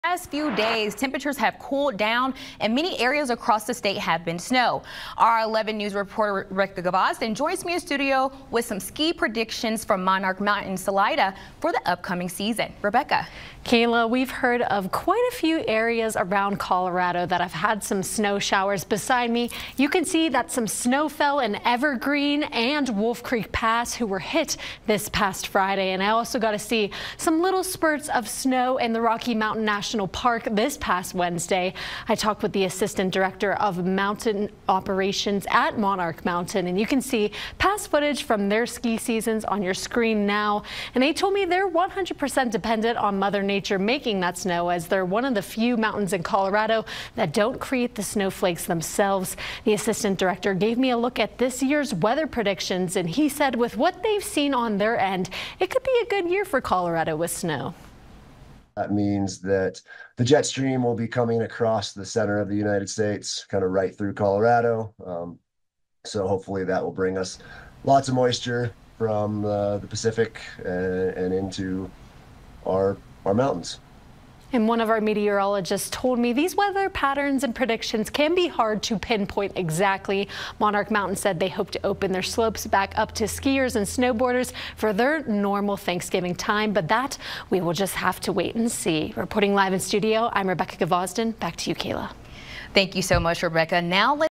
Monarch Mountain hopes to open slopes for Thanksgiving time: No Name Expansion Project in the wor...
Monarch Mountain relies solely on the natural snowfall and is one of the few mountains in Colorado that doesn’t make these snow piles themselves.